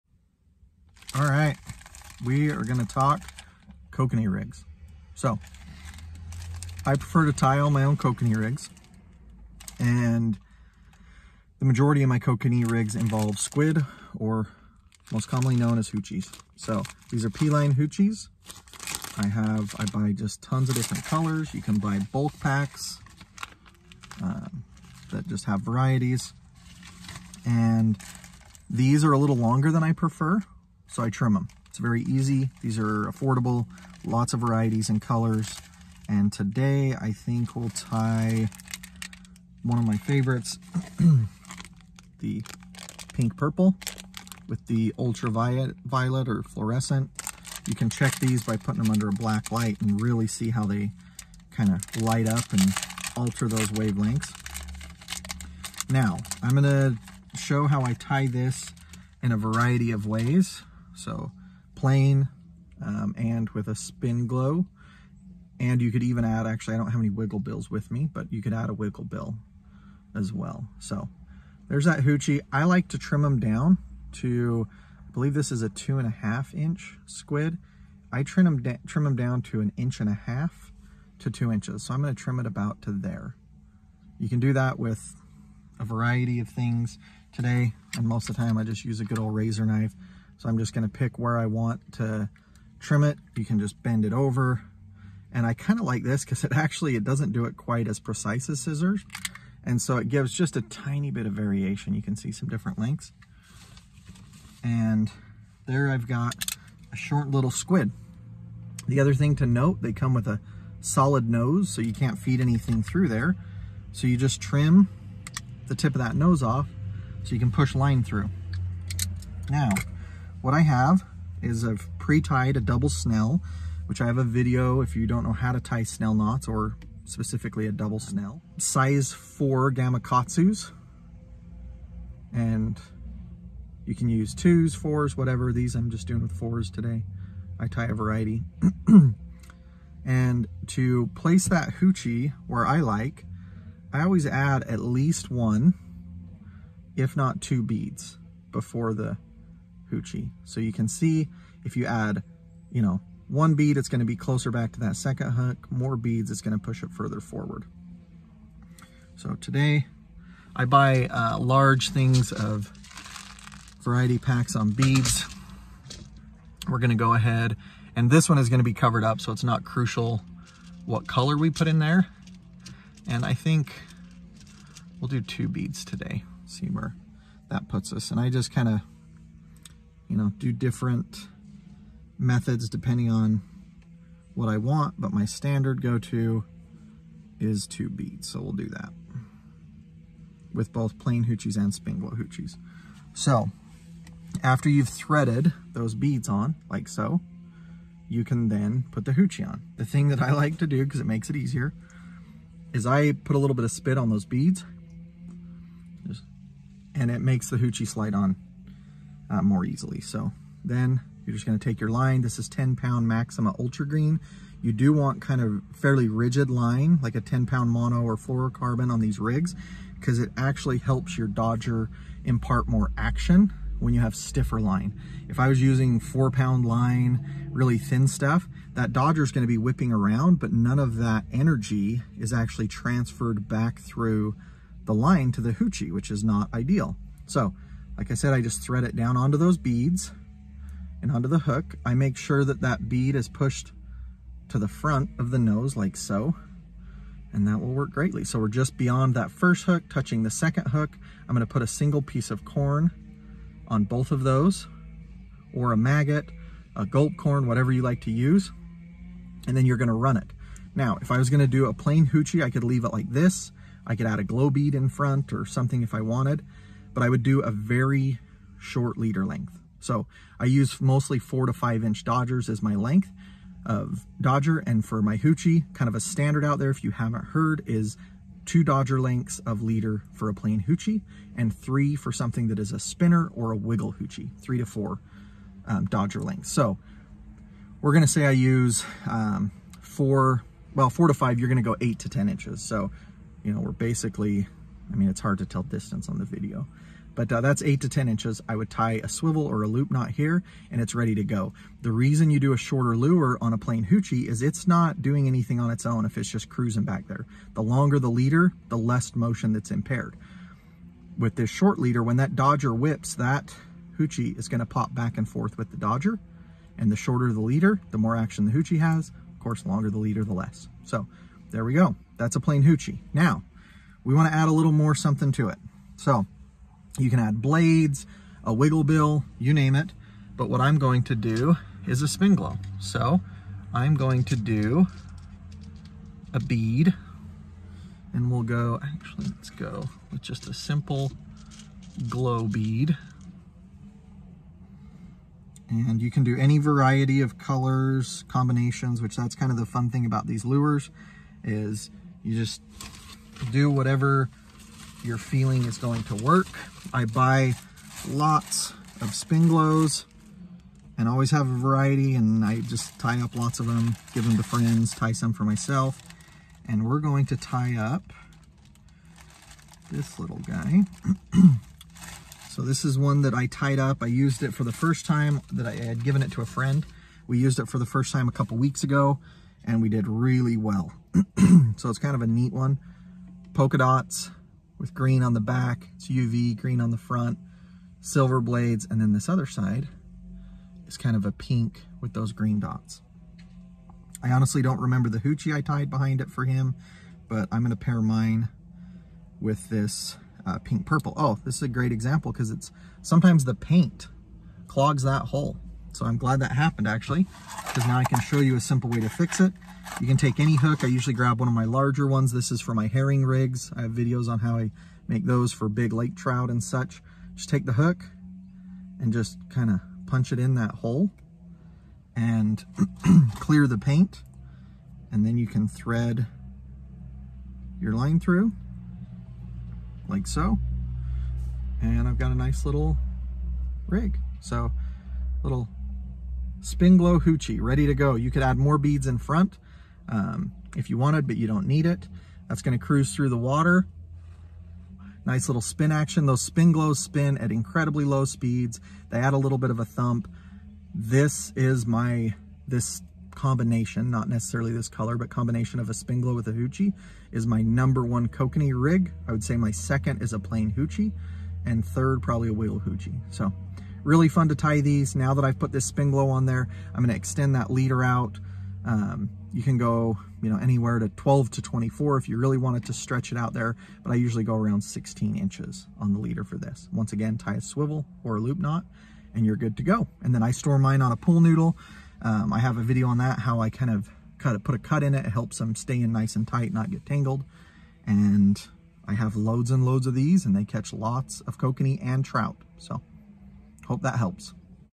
How to tie your own Kokanee lures
Catch loads of big Kokanee on your own rigs. Fun to tie, effective, and affordable!